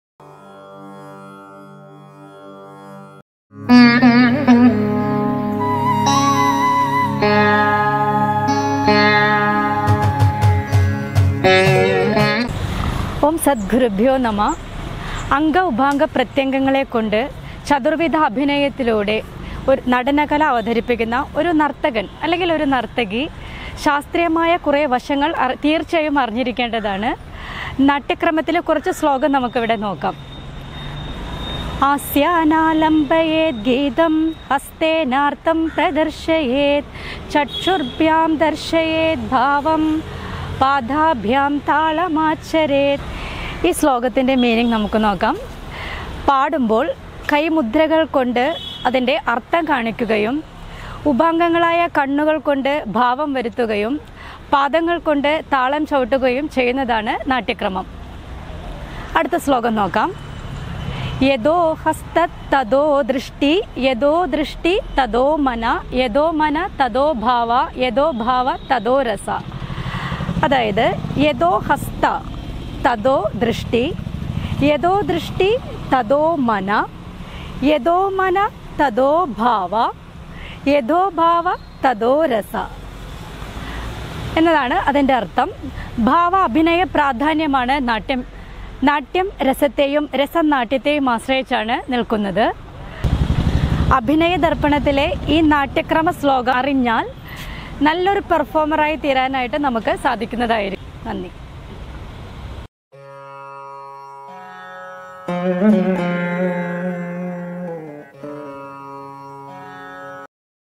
KRISHNA NRITHA KALAKSETHRA | നാട്യക്രമം | YATHO HASTHA THATHO DRISHTI | 2021
ഭാവാഭിനയപ്രാധാന്യമാർന്നതാണ് നാട്യം. ഗീതത്തിലെ സാഹിത്യത്തിനൊത്ത് മുദ്രകളും ഭാവാഭിനയവും കാഴ്ചവയ്ക്കുന്നത് നാട്യത്തിലൂടെയാണ്.

Anchor -Kalamandalam Jayalakshmi
Camera,Direction -Hari  Thiruvizhamkunnu